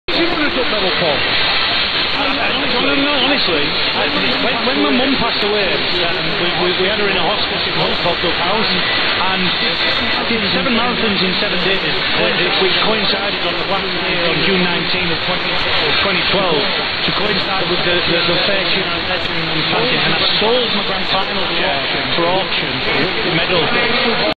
1089AM TalkSPORT (UK) 3100km 19:09 UTC 24-Sep-2022
1089AM TalkSPORT (UK)  3100km 19:09 UTC 24-Sep-2022
Multy TX from UK on 1089 kHz.
SDR RX-888MKII, WellGood loop V3